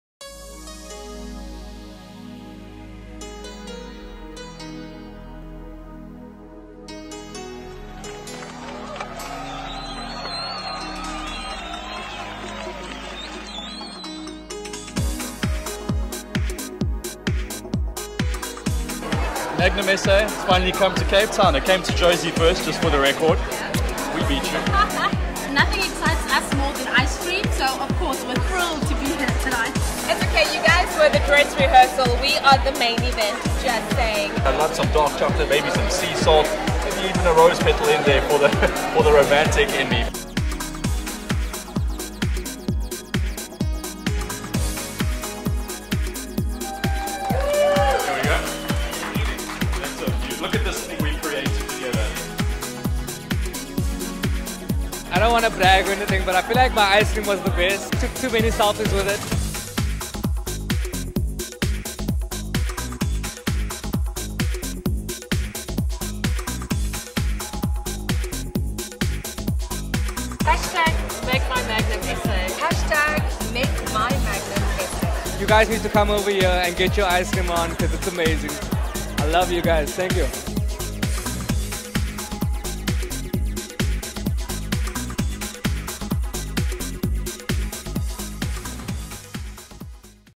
Magnum Cavendish Store in Cape Town
Join us on an indulgent adventure at the launch of the Magnum Cavendish store in Cape Town.